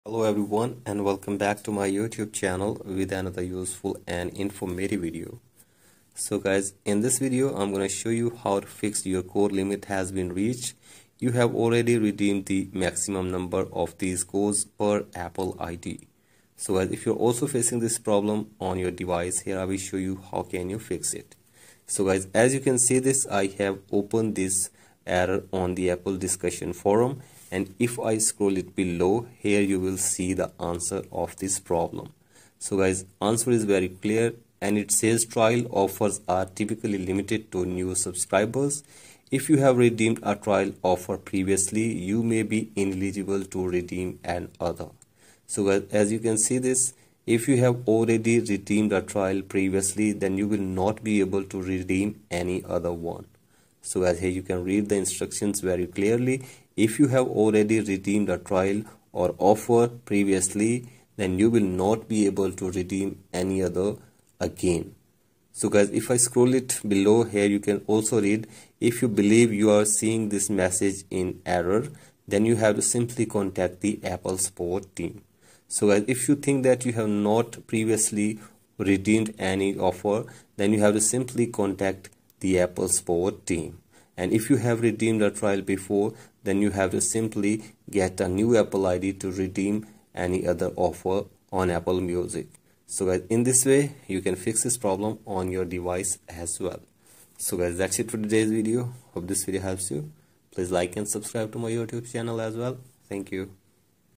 Your code limit has been reached Apple Music | 2024
Hi everyone,
in this video you will learn How to fix Your code limit has been reached issue in Apple Music.